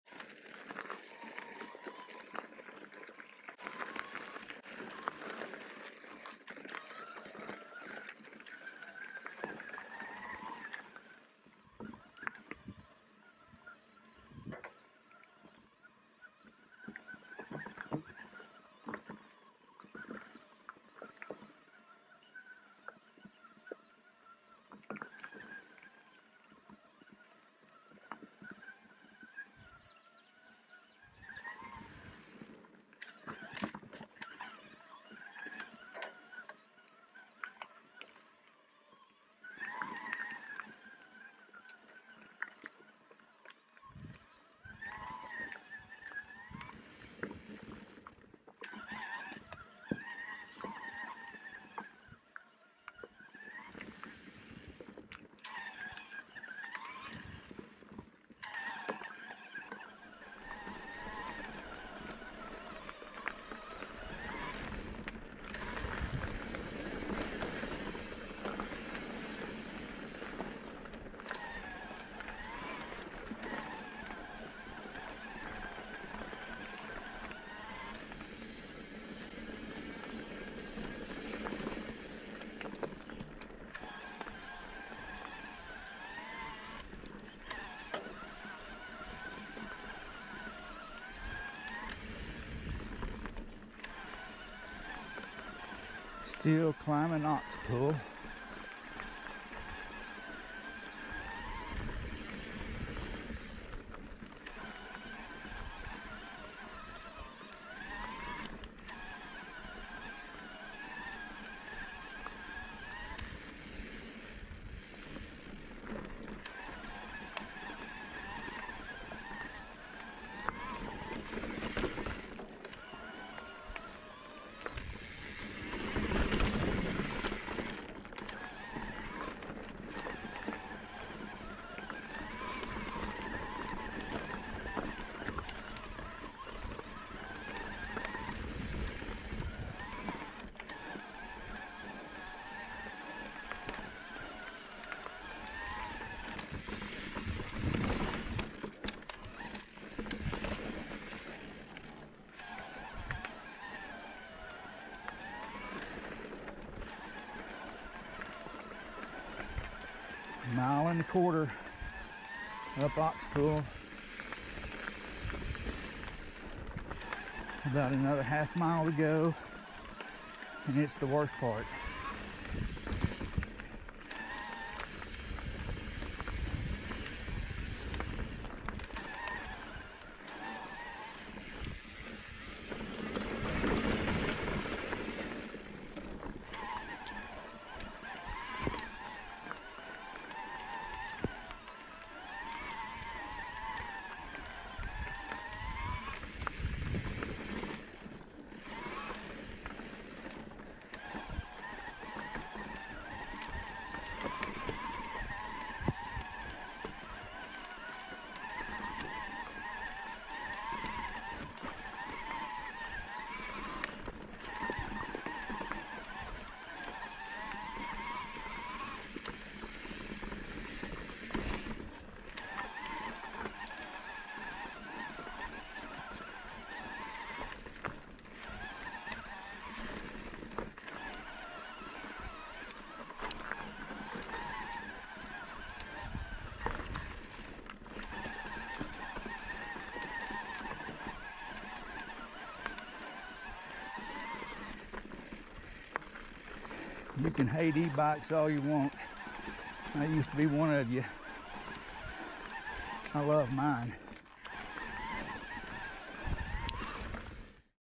Ox Pull Trail at Mount Nebo. Mountain Bike Trail
This is a one way trail, its a climb from the lower parking lot that connects with Millers Goat trail